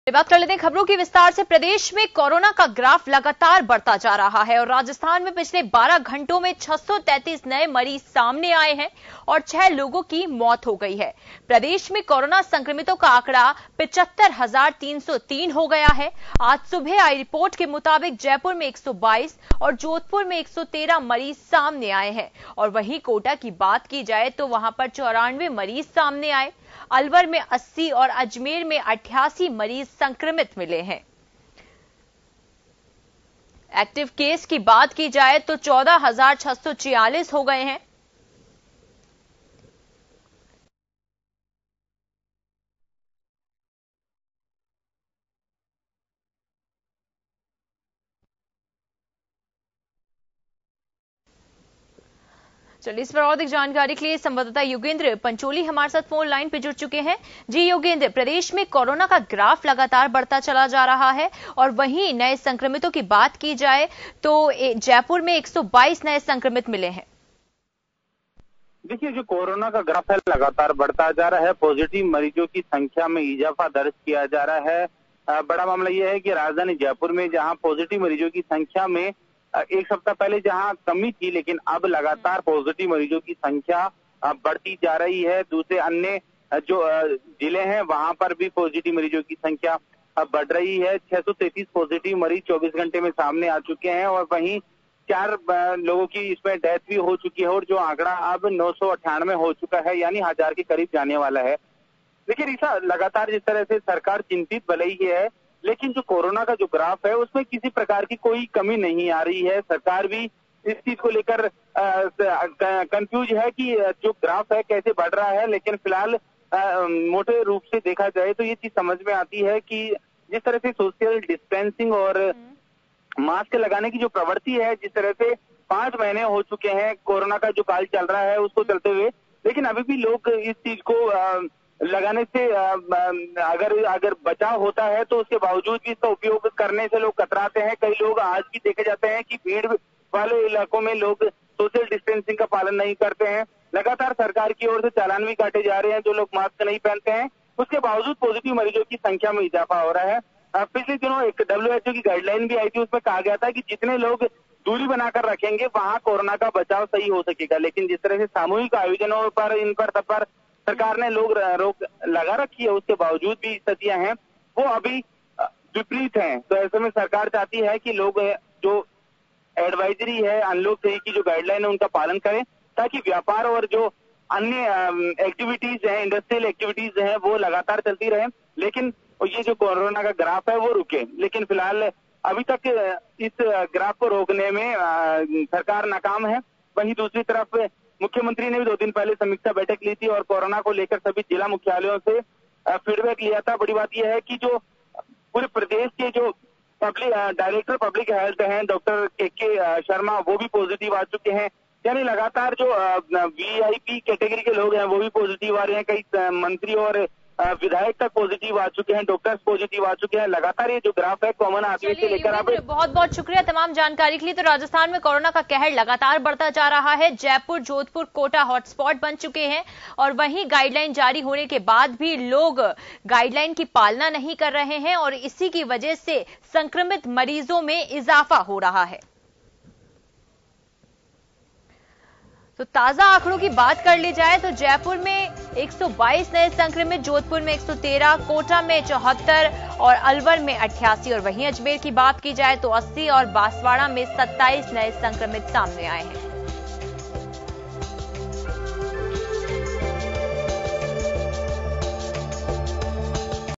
प्रदेश में बढ़ रहा है कोरोना का ग्राफ, 12 घंटे में 633 नए मरीज, Jaipur, Jodhpur, Kota हॉट स्पॉट
News credit : rh